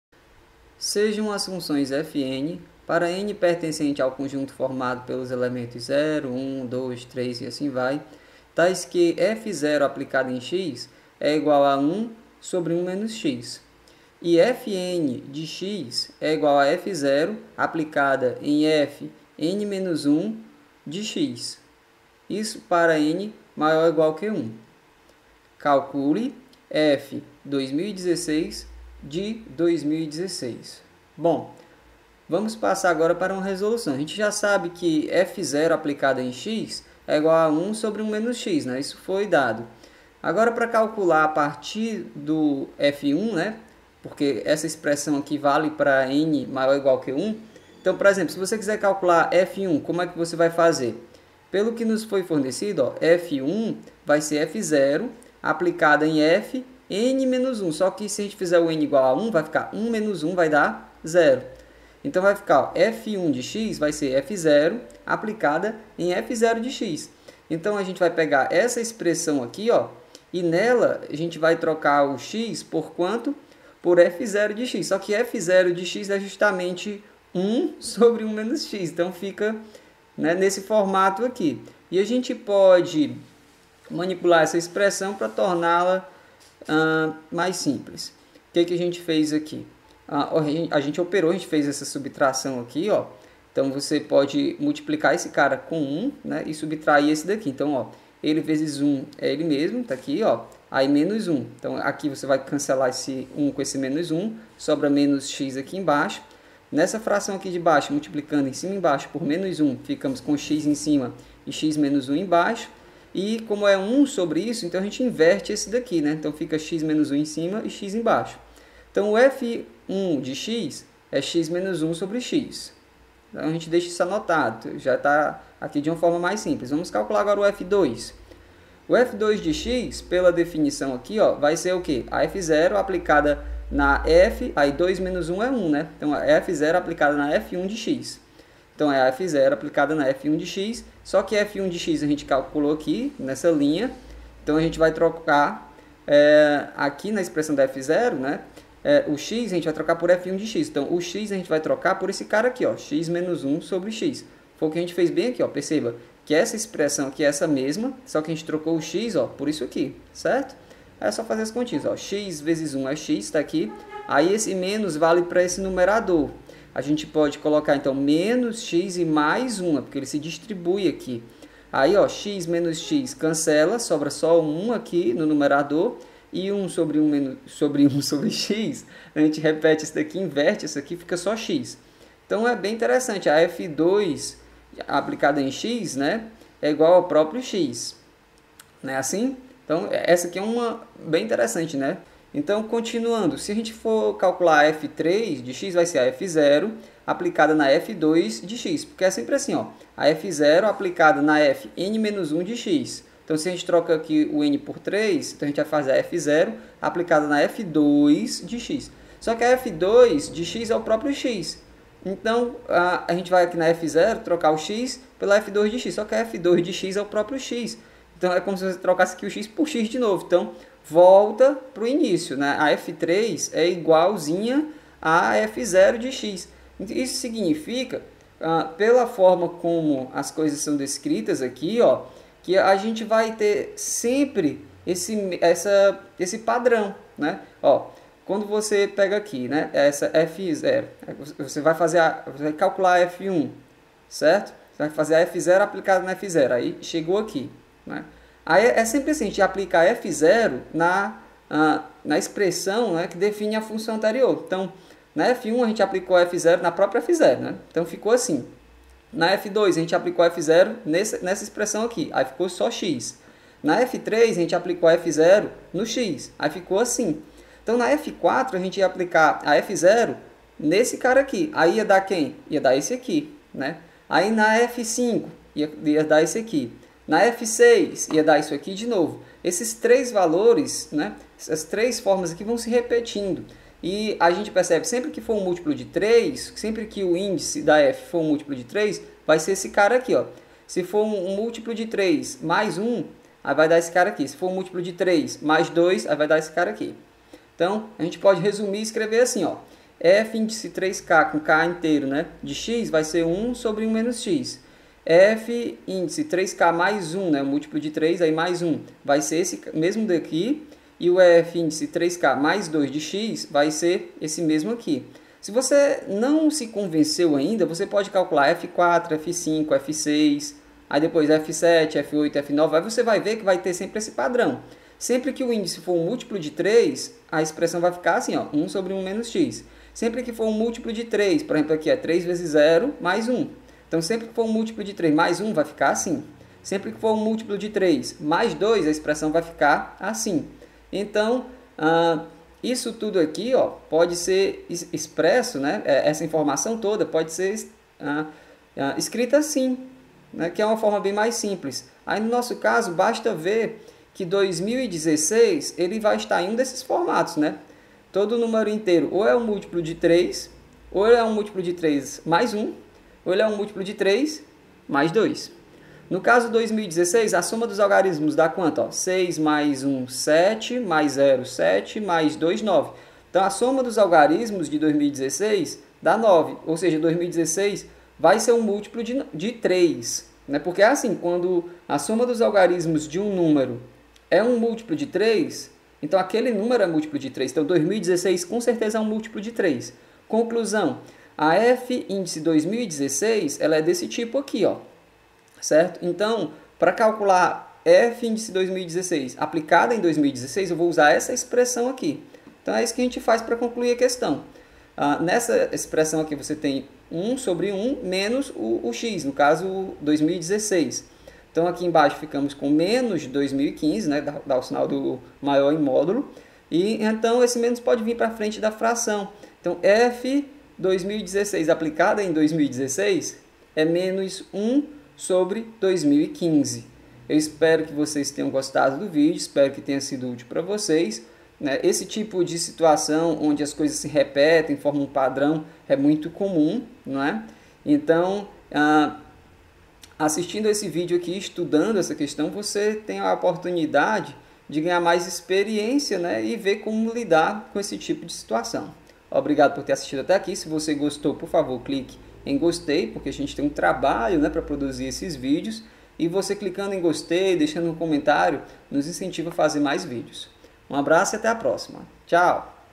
Função Composta - IME 2015/2016 - Discursiva - Questão 2
Sejam as funções f_n, para n \in {0,1,2,3,...}, tais que f_0(x) = 1/(1-x) e f_n(x) = f_0(f_{n-1}(x)), para n \geq 1. Calcule f_{2016}(2016)